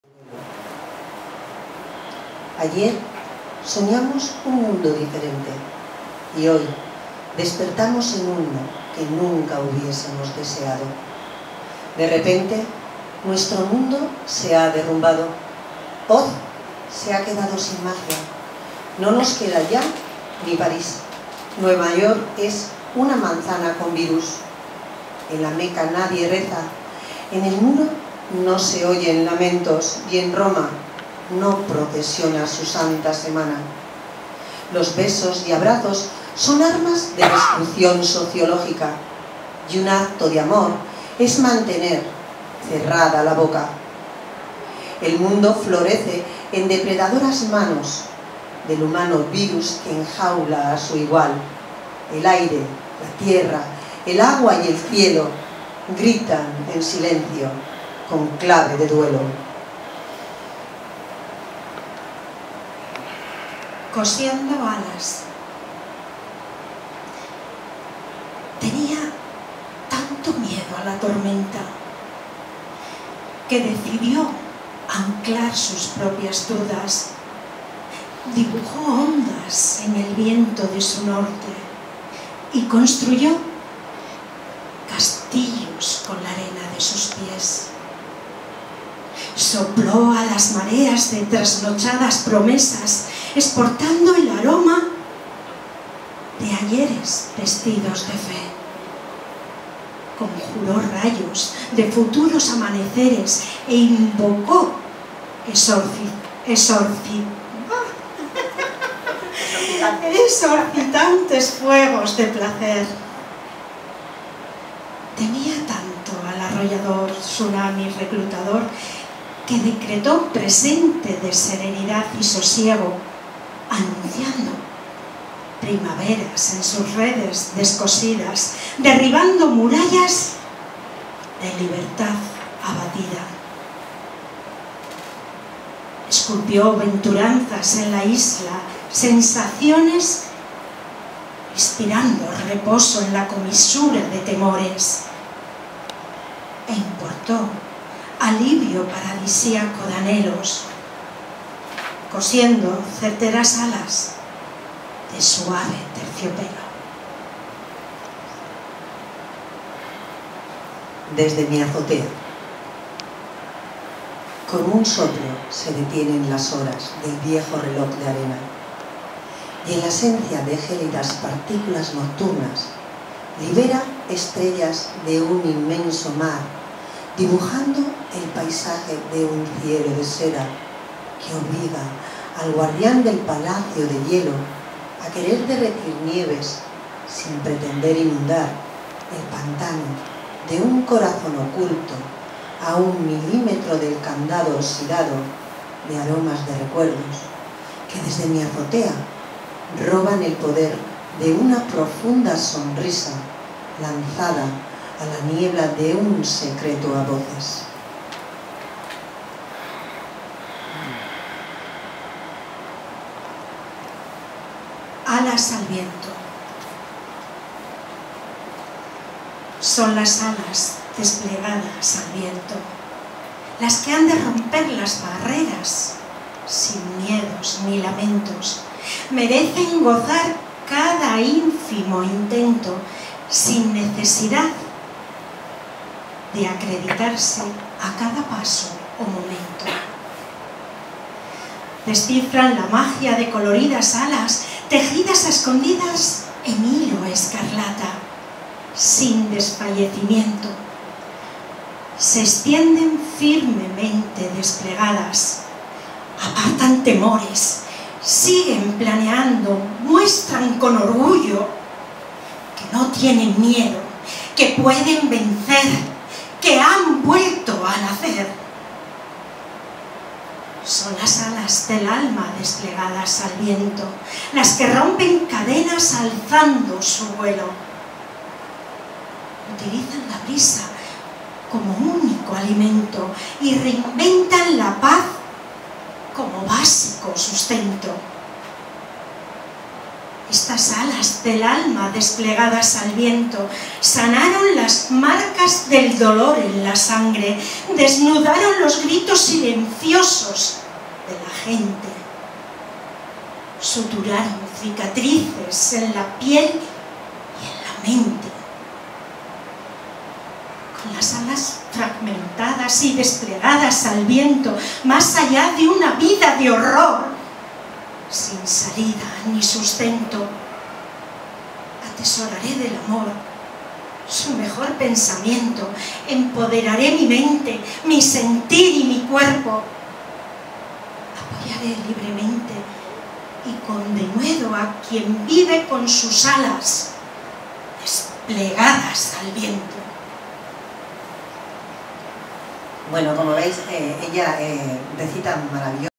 Presentación del libro : Cuando las miradas gritan de Dory Lansorena 9 Diciembre 2021 ( 4 )
Presentación en Fnac - Bilbao del Libro de Dory Lansorena "Cuando las miradas gritan".